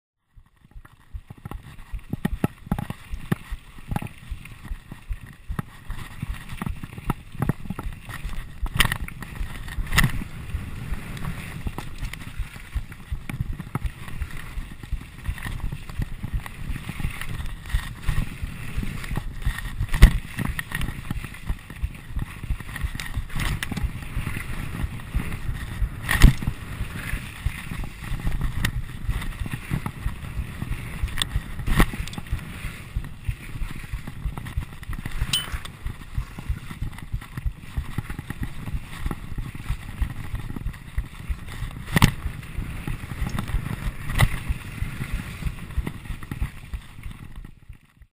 GoPro - Sawmill Trail - Stornoway - 2013
GoPro of the the mountain biking trail at the sawmill. Giving it some raj.